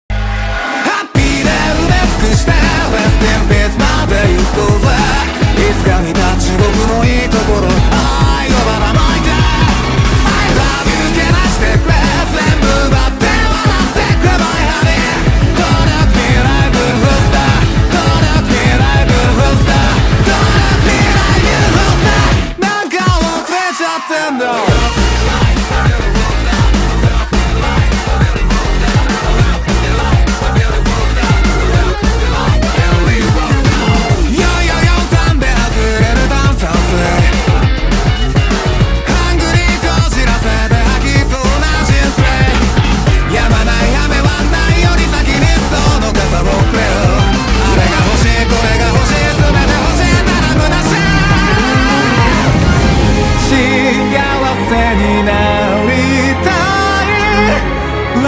Kymco Dtx360 my land-jetski😅
Motorcamping Calatagan Batangas Para sa Sunset

Camp site is at Edna's beach Calatagan Batangas.
Ok naman for camping yung lugar hindi sya for swimming kasi muddy yung area at maliit na portion lang yung mostly sand. Ok din yung CR nila, malakas yung tubig at somehow madami din.
Siksikan yung yung camp site kasi ang daming tao pag week end talagaf kaya nyo naman pumunta ng weekdays much better. So over all experience ok naman sya sa amin kasi nga ang pinunta naman talaga natin dito is "PHOTOGRAPHY". Again, hindi sya good for swimming ah, pero pwede naman depende sa trip nyo